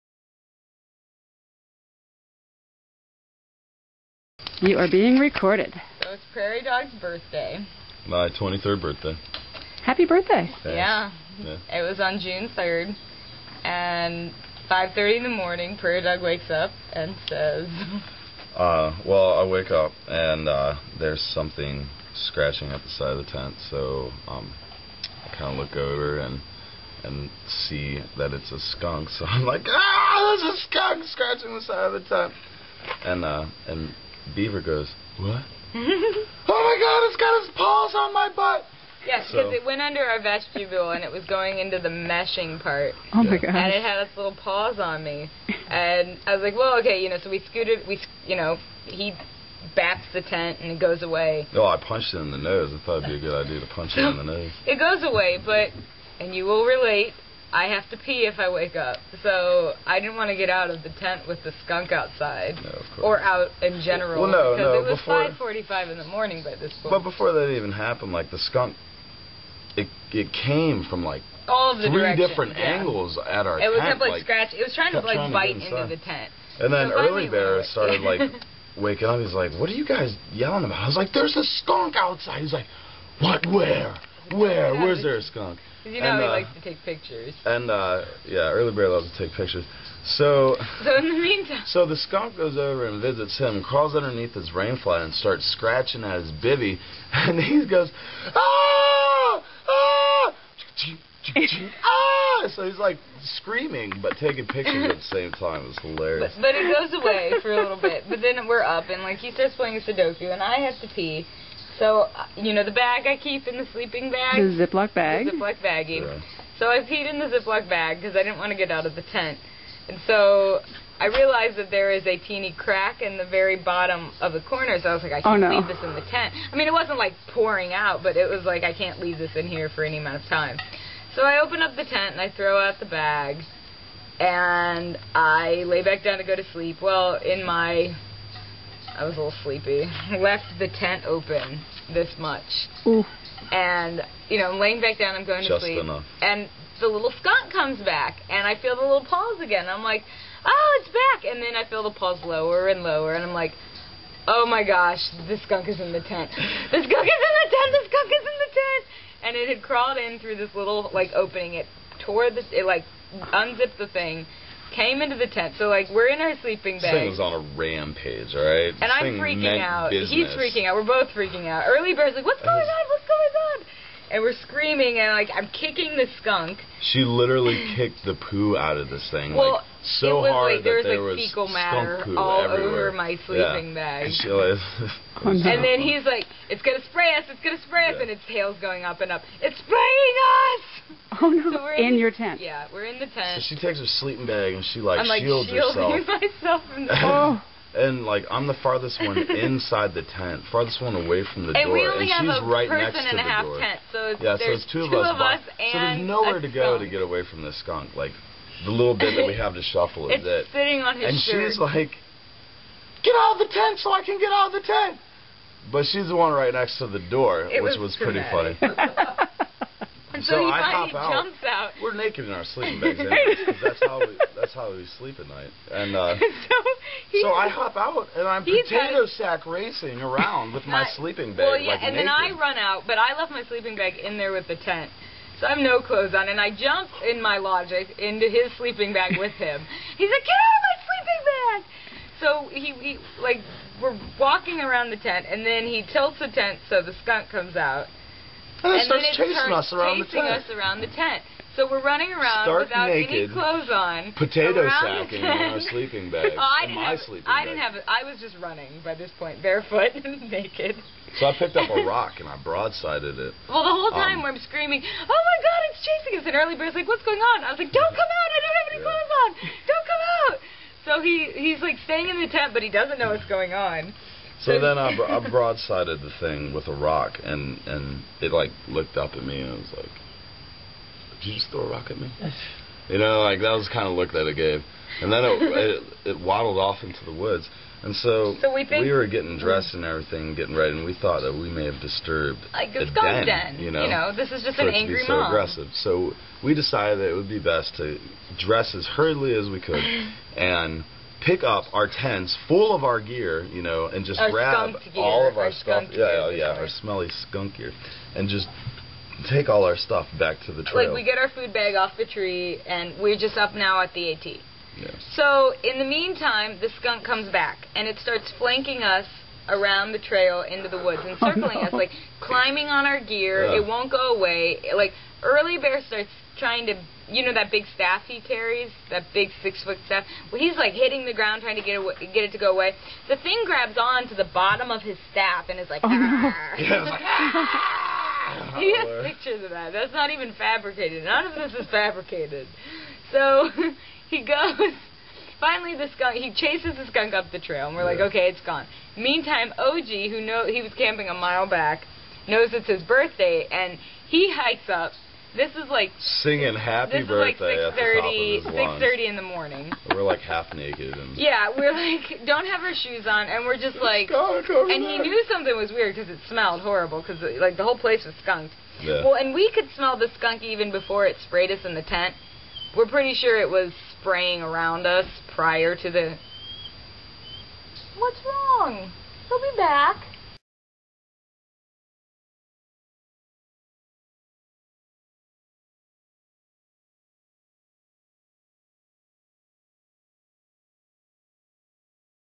Skunk Story Part 1 Movie
Angry Beaver and Prarie Dog telling the story about a rabid skunk attacking them and Early Bear during their 2009 Appalachian Trail Thru-hike.  This is Part 1.  The story continues in Part 2.